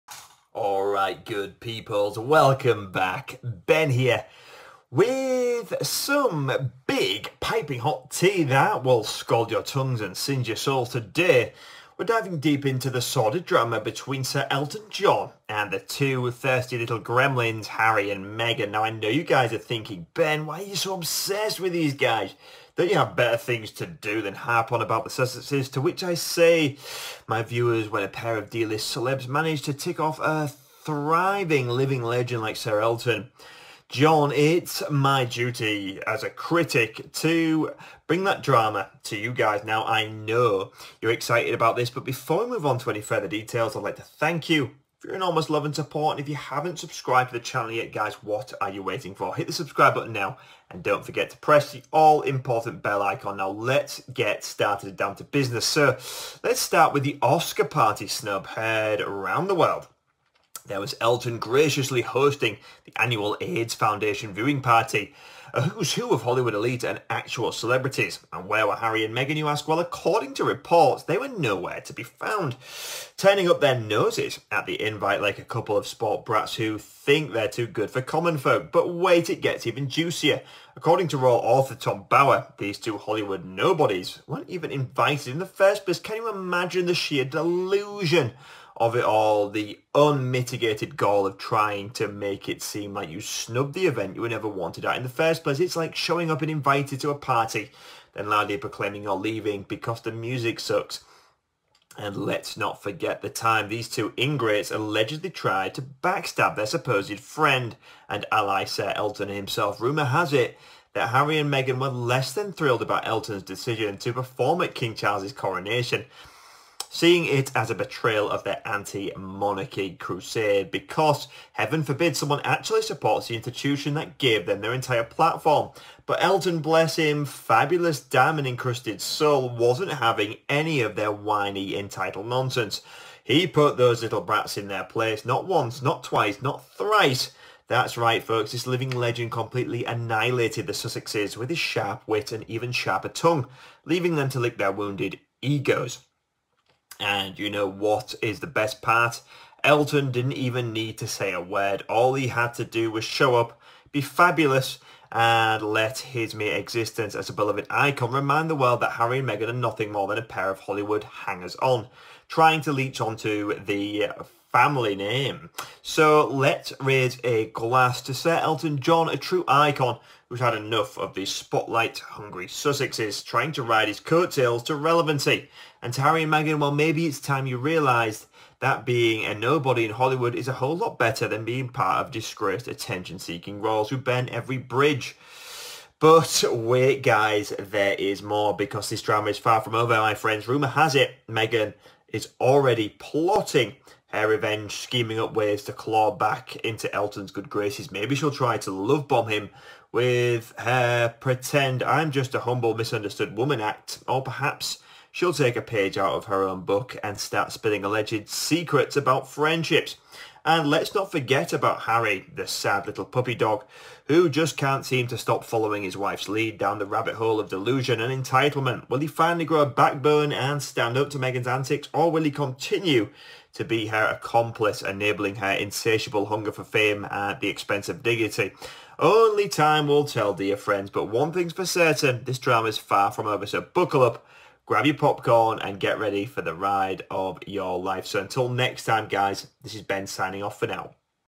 IT'S ALL OVER BETWEEN US! Furious Elton John's DESTROY EV!L Harry & Meghan With Brutal 'OSCAR SNUB'.
"🎉 Elton John's Oscar Party Snub: Royal 'Nobodies' Harry & Meghan Left in the Dust! 😱 The Epic Showdown: Hollywood Icon vs. Sussex Spoiled Brats 🌟 Sir Elton's Fabulous Fabulousness Strikes Again! 💅 Harry & Meghan's Backstabbing Betrayal Exposed 🔪 Meghan's Revenge Plot Unveiled: Will She Claw Her Way Back In? 🤔 Harry's Dilemma: Stand Up or Stay Silenced? 🐶 The Saga Continues: Hollywood Drama at Its Finest! 🍿 EltonJohn OscarParty HarryandMeghan RoyalDrama HollywoodNobodies FabulousElton RevengePlot TeaTime PettyDrama"